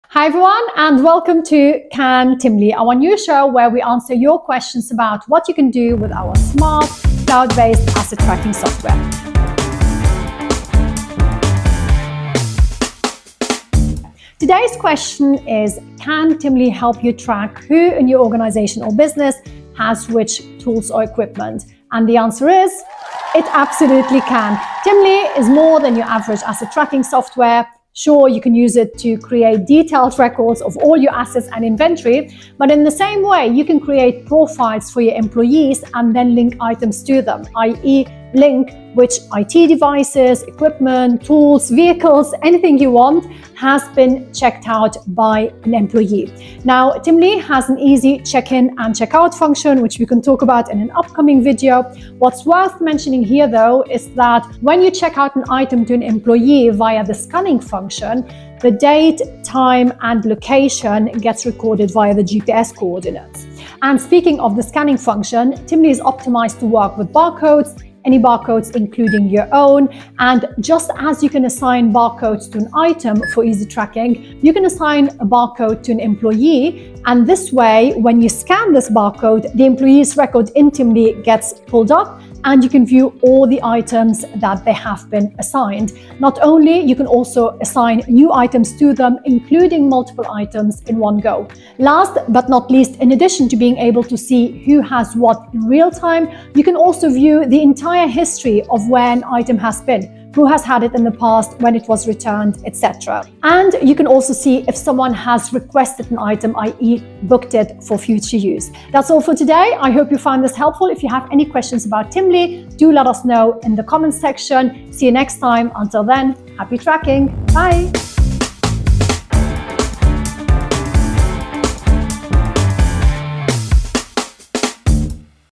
Can Timly...? #1: Who Has Which Assets
Hi Everyone! 👋 Welcome to "Can Timly...?", our brand new show where we answer your questions about what you can achieve with asset tracking software. 🧐

In this first episode, we're tackling a frequently asked question: How can you use Timly to keep track of who in your business or organisation is using which tools or equipment? 🛠️🔍

Did this video hit the mark and answer your query? Also let us know if you have any other questions - we'd love to feature them in an upcoming episode. 🏷️✨